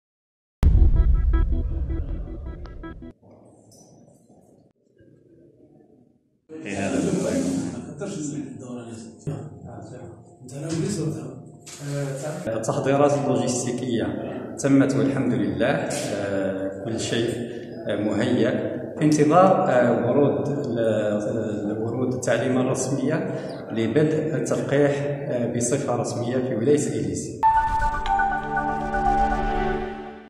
إيليزي - كورونا : ضبط كافة الترتيبات تحسبا لعملية التلقيح
إيليزي (وأج) - تم ضبط كافة الترتيبات والإمكانيات اللوجستية على مستوى المراكز والوحدات المخصصة لعملية التلقيح ضد كوفيد -19 بولاية إيليزي، والمرتقب الشروع فيها خلال الأيام القادمة.